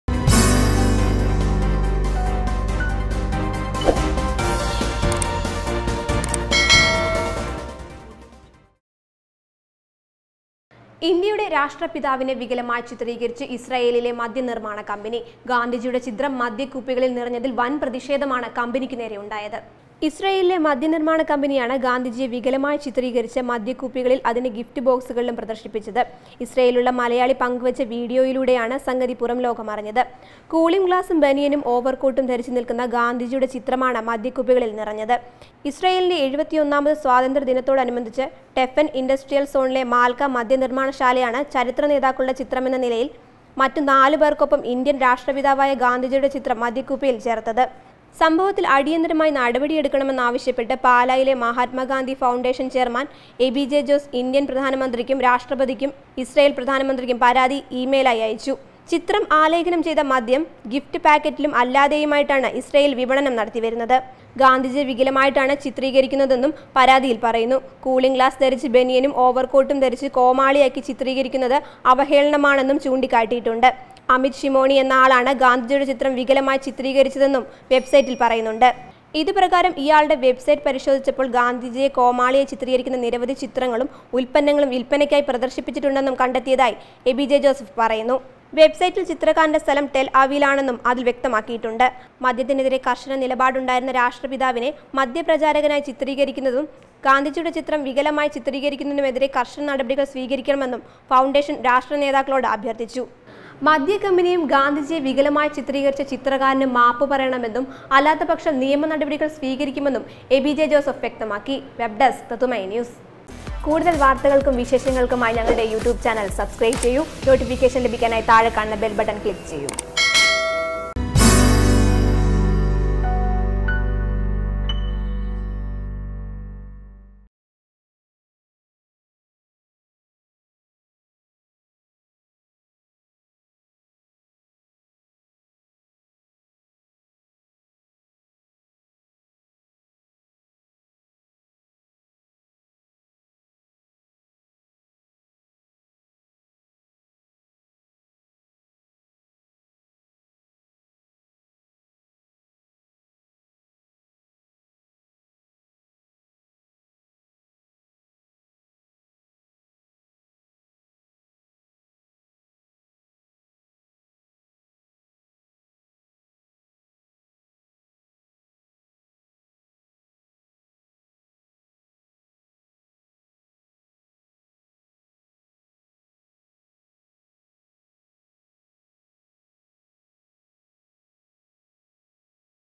രാഷ്ട്രപിതാവിന്‍റെ ചിത്രം മദ്യക്കുപ്പികളിൽ; കമ്പനിക്കുനേരെ വന്‍ പ്രതിഷേധം
ഇസ്രലേയിലെ മദ്യ നിർമ്മാണ കമ്പനിയാണ് ഗാന്ധിജിയെ വികലമായ ചിത്രീകരിച്ച് മദ്യക്കുപ്പികളിലും അതിന്റെ ഗിഫ്റ്റ് ബോക്‌സുകളിലും പ്രദർശിപ്പിച്ചത്. ഇസ്രയേലിലുള്ള മലയാളി പങ്കുവെച്ച വീഡിയോയിലൂടെയാണ് സംഗതി പുറംലോകമറിഞ്ഞത്. കൂളിങ്ഗ്ലാസും ബനിയനും ഓവർക്കോട്ടും ധരിച്ച് നിൽക്കുന്ന ഗാന്ധിജിയുടെ ചിത്രമാണ് മദ്യക്കുപ്പികളിൽ നിറഞ്ഞത്.